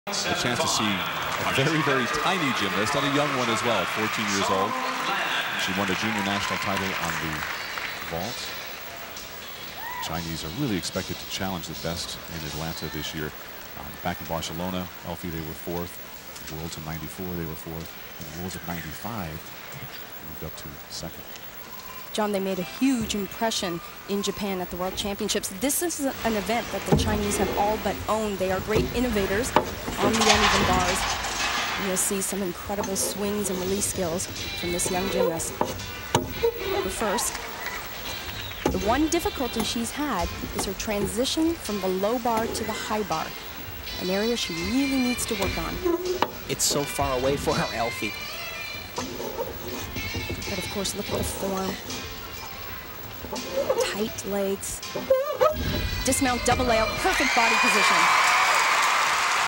Sang Lan - Uneven Bars - 1996 McDonald's American Cup
Score: 9.362. Sang Lan (China)
March 2, 1996 - Tarant County Convention Center - Ft. Worth, Texas